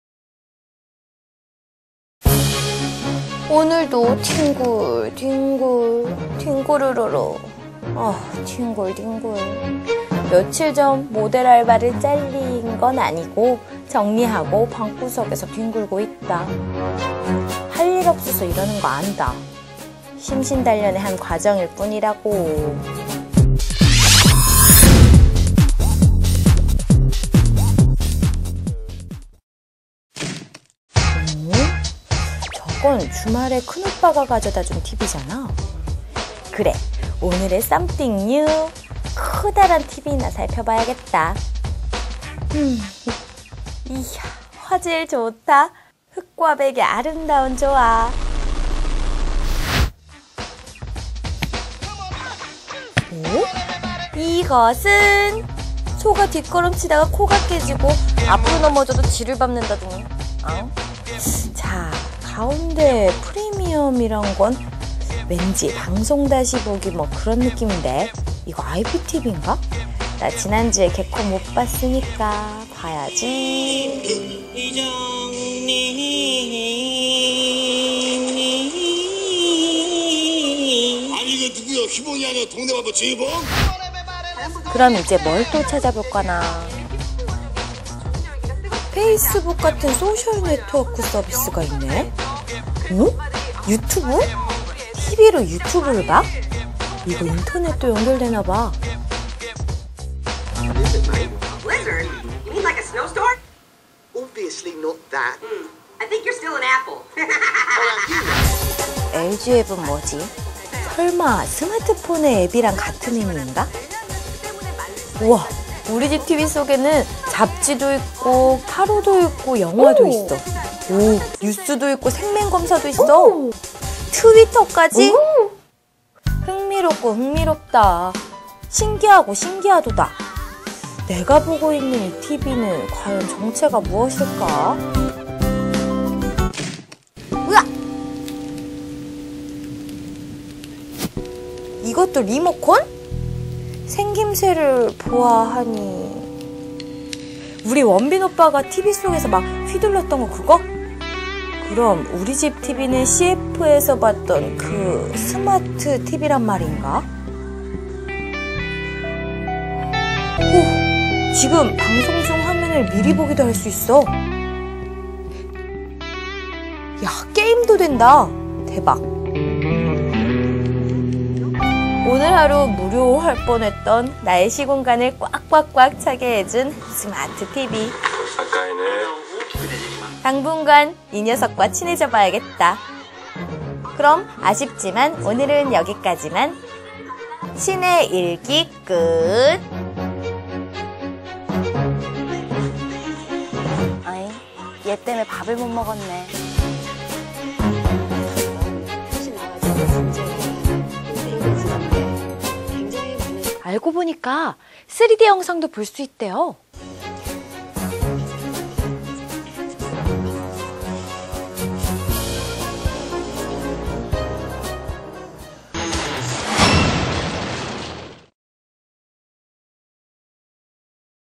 IPTV? 앱설치? 스마트TV? -시내의 일기-
가운데 프리미엄~이란 건~ 왠지 방송 다시 보기.. 그런 느낌인데 이거 iptv...인 건가? 그래, Kbs가 있네? 나 지난 주 개콘 못 봤는데~ 페이스북 같은 소셜~네트워크~서비스~가 있네? 앗 유투브~ TV로 유투브를 본다고? 흥미진진한데~ 이거 인터넷도 연결되나보다. 흠~ 옆에 LG앱은 뭐지?